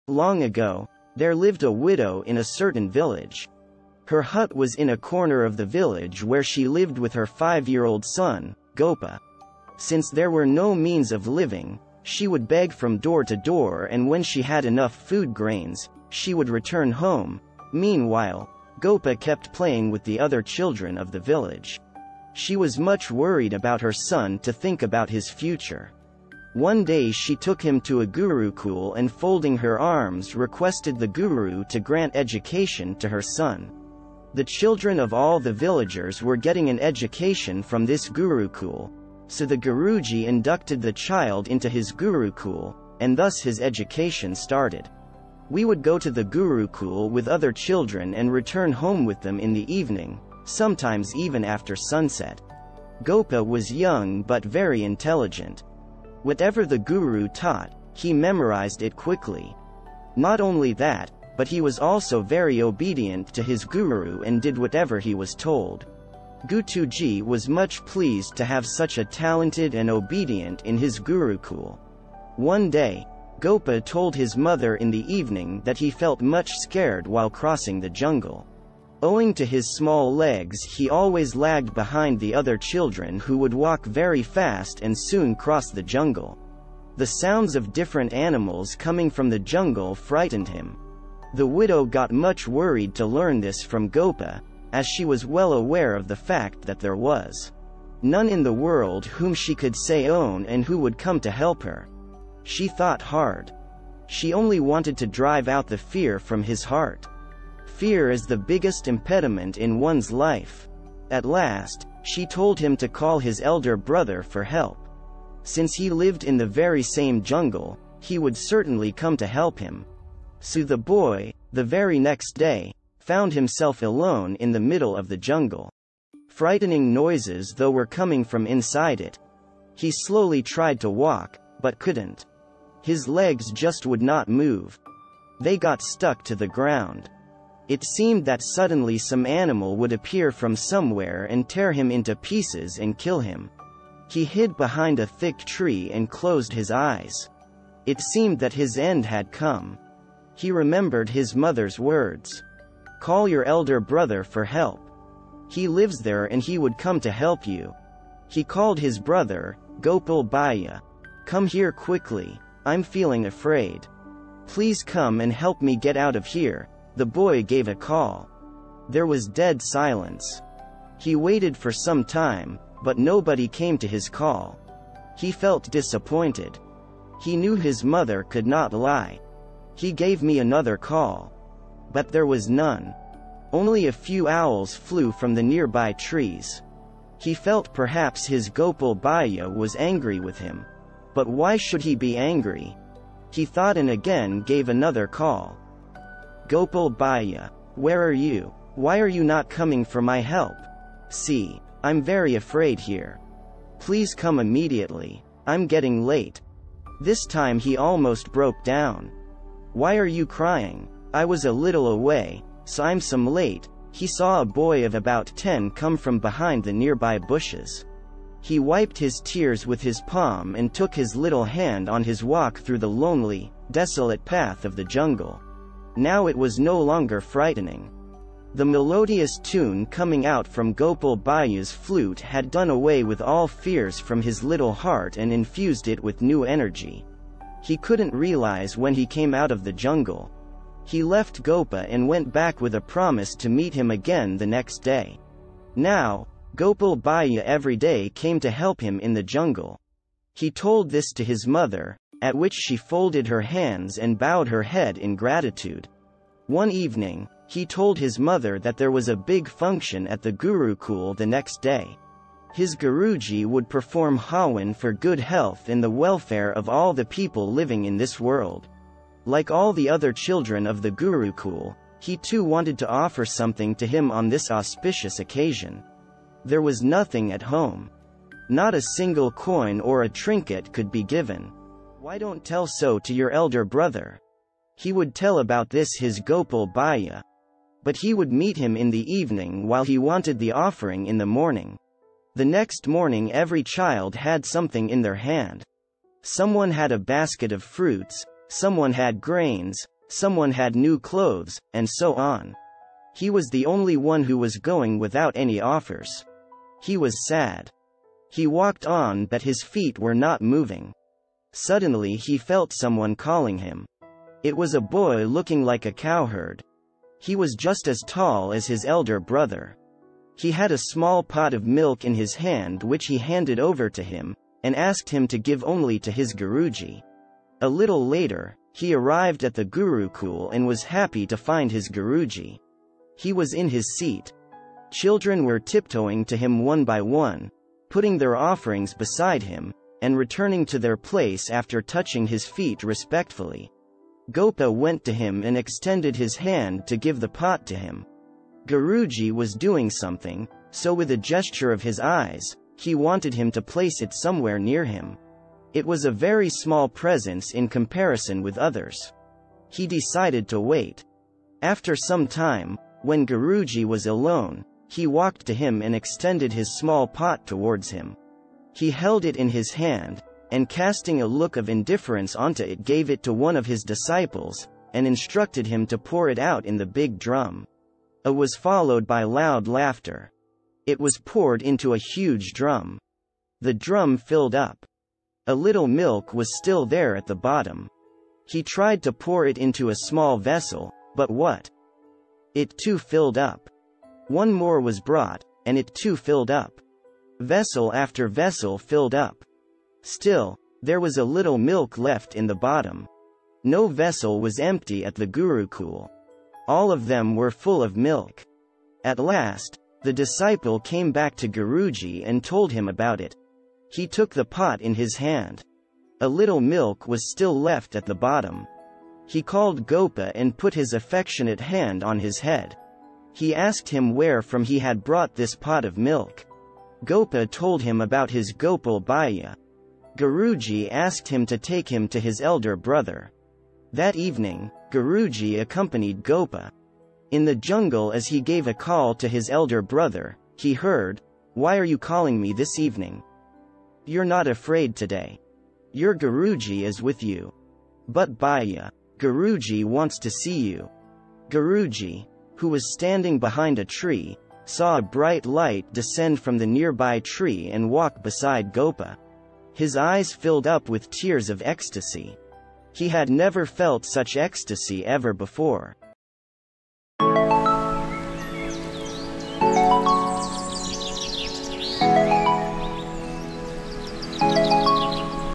"Facing Your Fears: Gopa's Jungle Adventure" | Very Interesting story
The video starts with a panoramic view of a beautiful and serene countryside. As the camera zooms in, we see a group of young hikers on a trek, laughing and chatting with each other as they make their way through the picturesque terrain.

As they continue on their journey, they stumble upon an abandoned village nestled amidst the hills. Intrigued by the ruins, they decide to explore the village and its mysterious history.

The video then takes us on a tour of the village, showcasing the crumbling buildings, the overgrown paths, and the forgotten relics. As the hikers delve deeper into the village, they uncover a haunting tale of love and loss that has been lost to time.

With stunning visuals and an engaging narrative, this video takes us on a journey through history and brings to life a forgotten village that time has left behind.